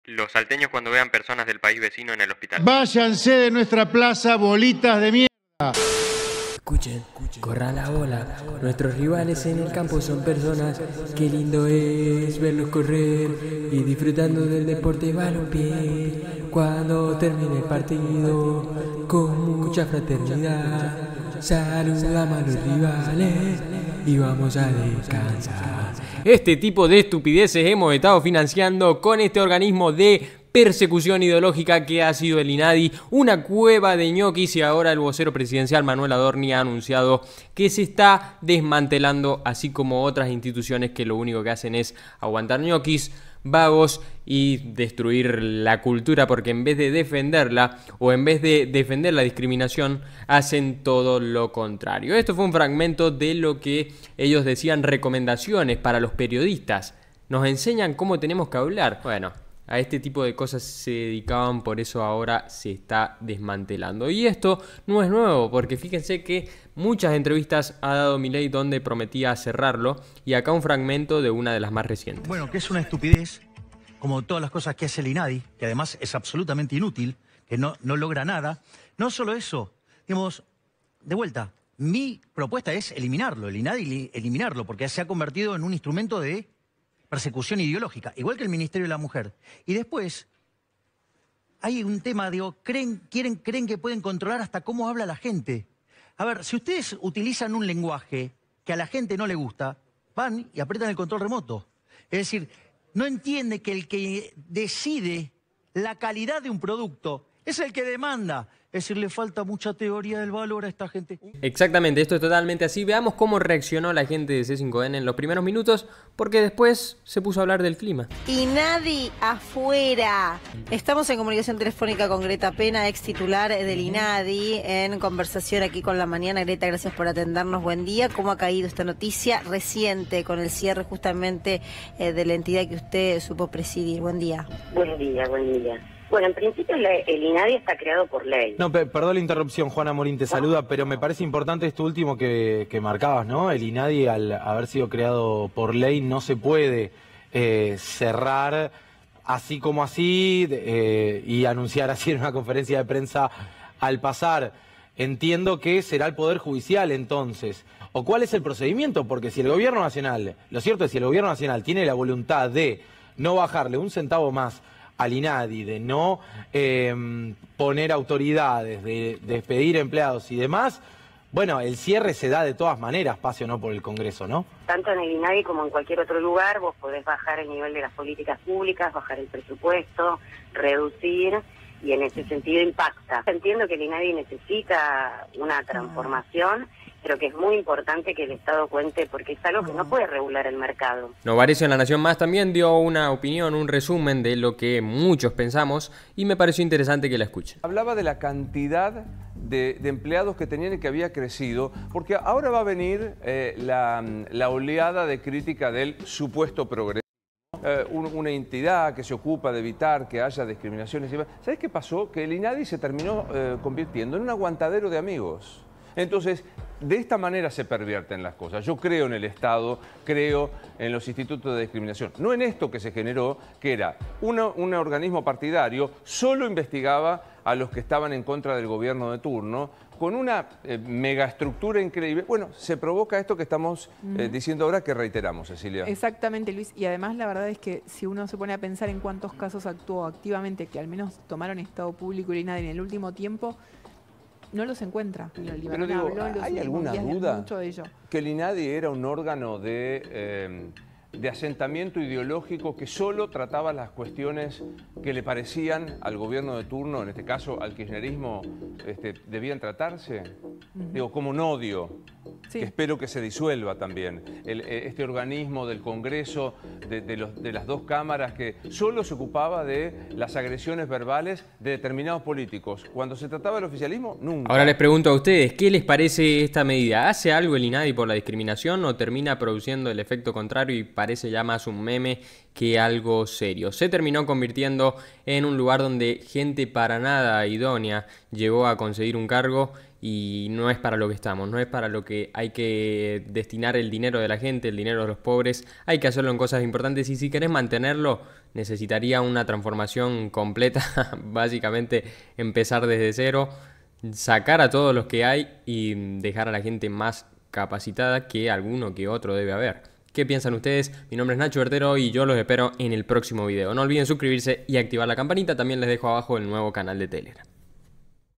😭 Periodistas ZURD0S LLORAN Por El CIERRE Del INADI - NO PUEDE SER ! 😭
Manuel Adorni anunció que se cierra el INADI

que opinás vos de esta medida? Mira en este video como reaccionaban
los periodistas de C5N, puede eliminarse asi tan facil?